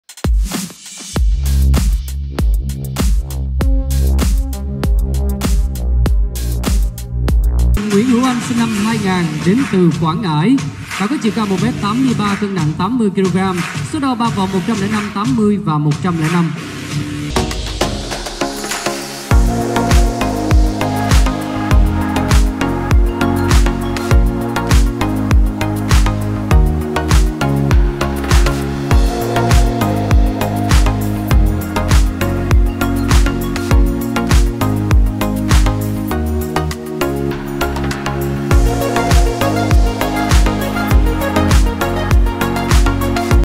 MAN OF THE WORLD VIETNAM 2022 | NGUYEN HUU ANH
He will compete in Man of the World 2022 as the reigning winner of Vietnam Fitness Model 2021
Good luck, man.